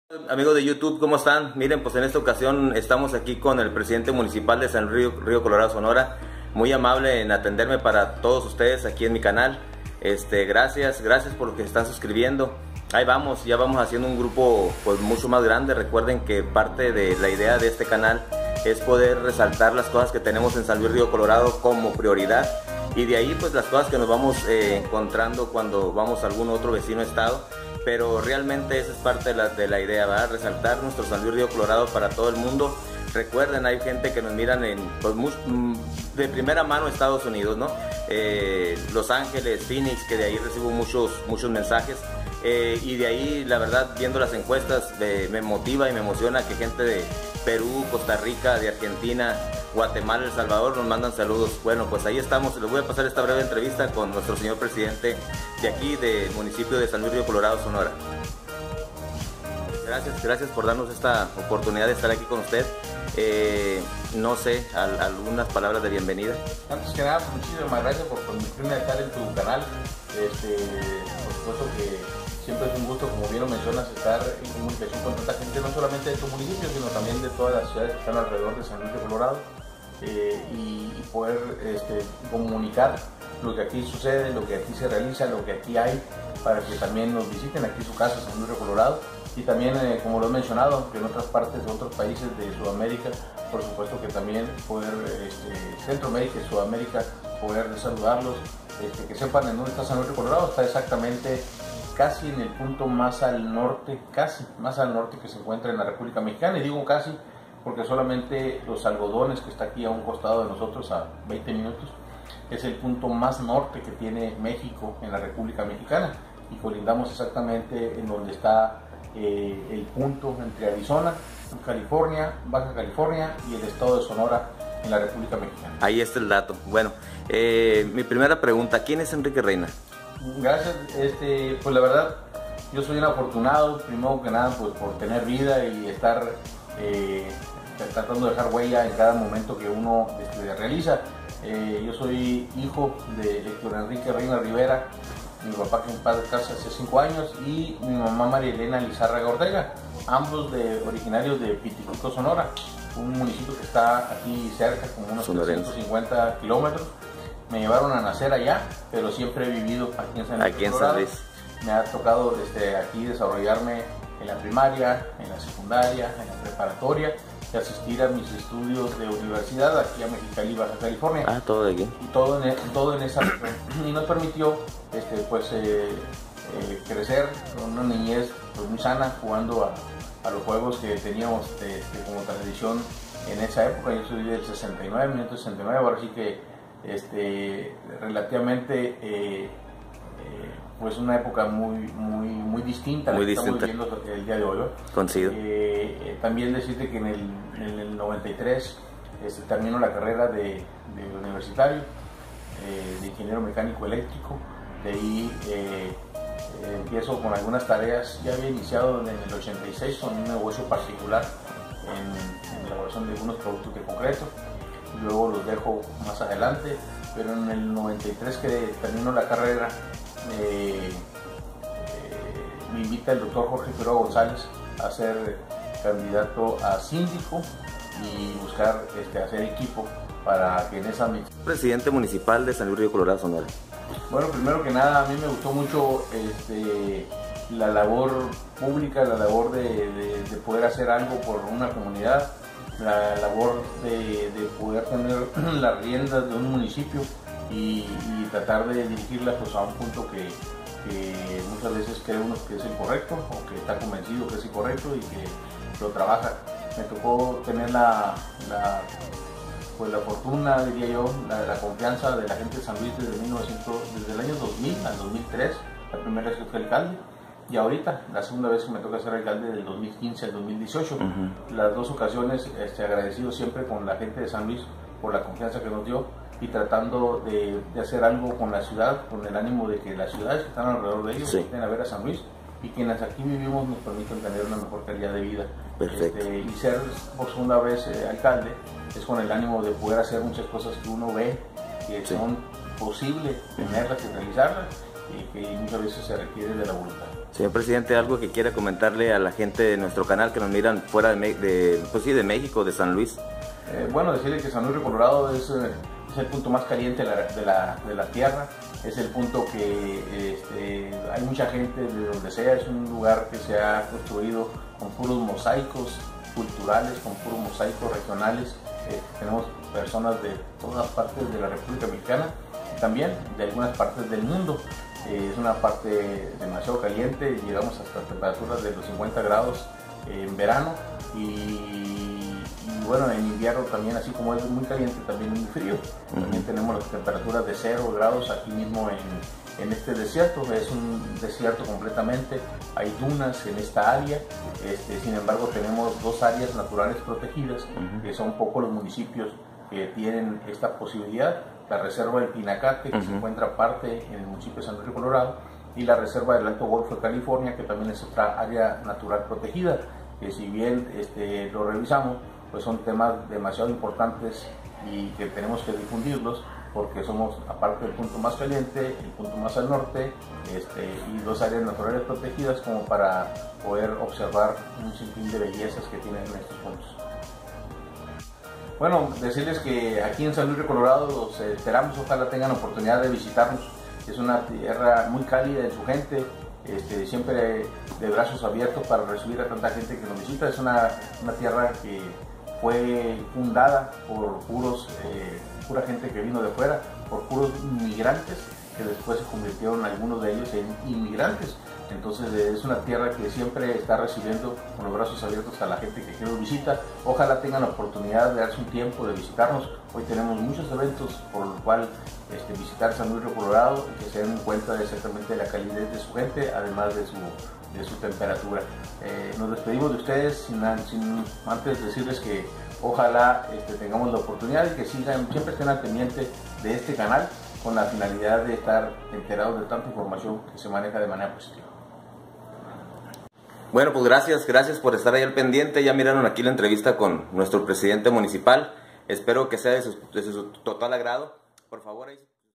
Enrique Reina Lizarraga alcalde de San Luis río Colorado sonora México
Una pequeña entrevista con el alcalde de nuestra comunidad antes de terminar su trienio. Saludos.